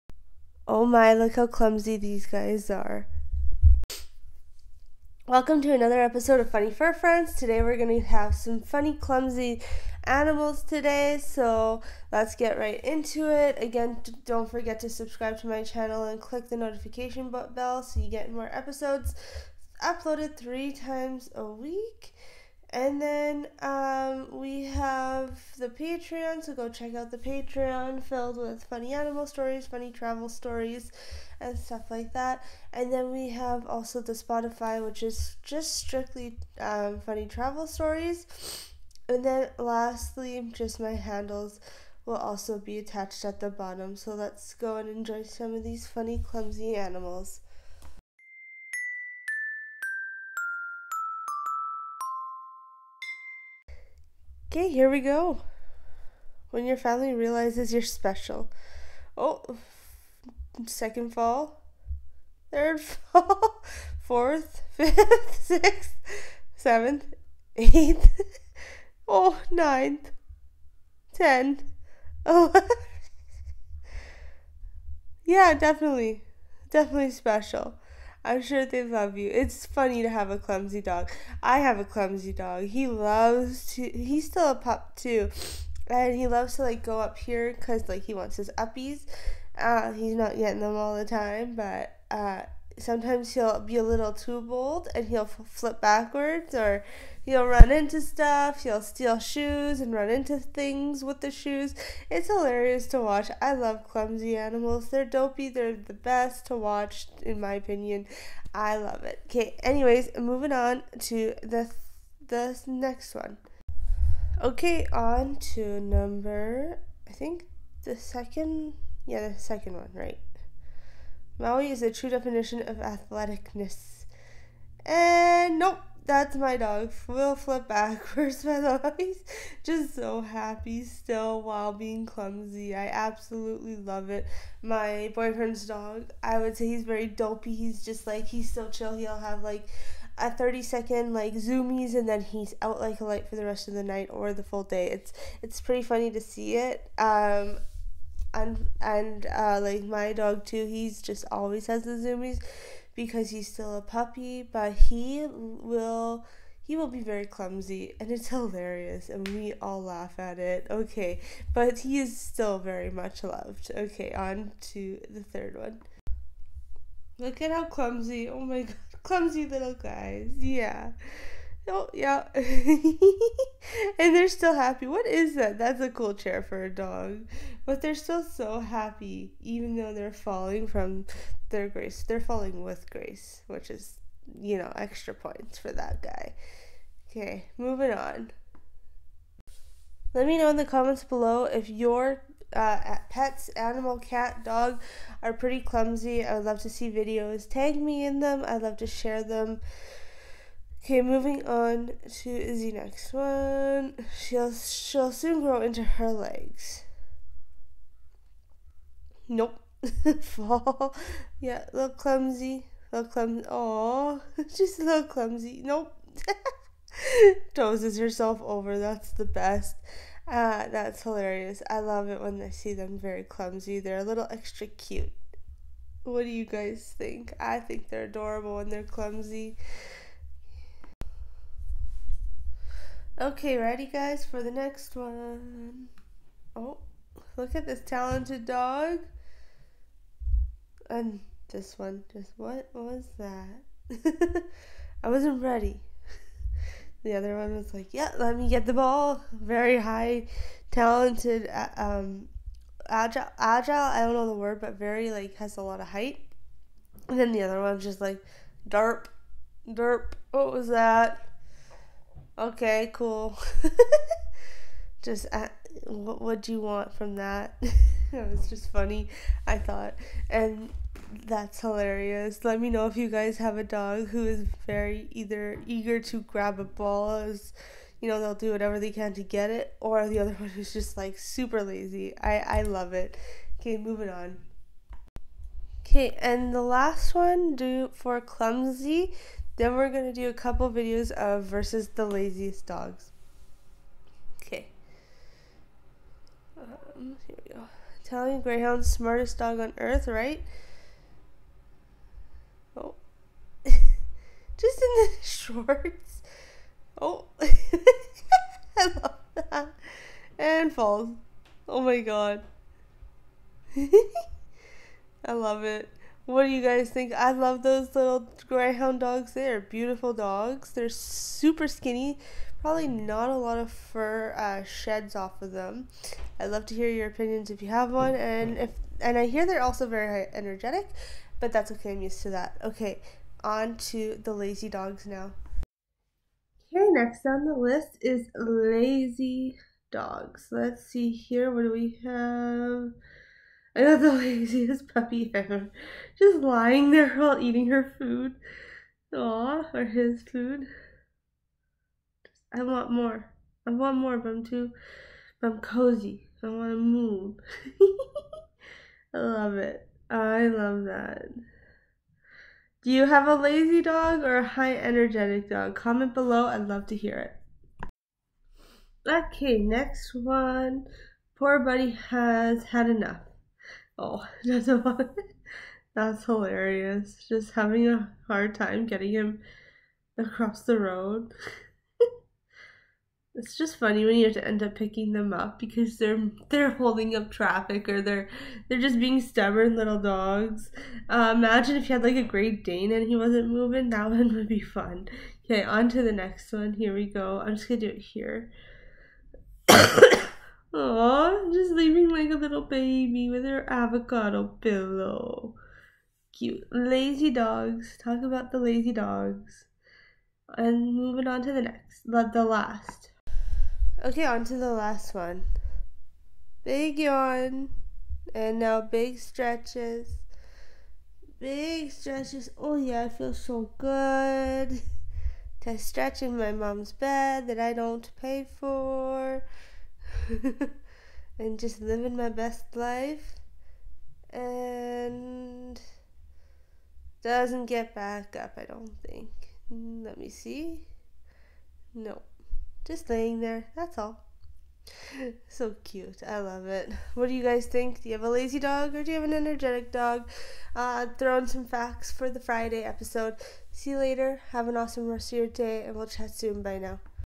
Clumsy Animals vs Lazy Animals - My Reaction
Welcome Funnyb Fur Friends

Watch my hilarious reaction as I compare clumsy animals to lazy animals! From silly falls to adorable laziness, these furry friends will have you laughing out loud. Which type of animal do you find more entertaining? Join me in this fun and lighthearted video to see who comes out on top - clumsy animals or lazy animals!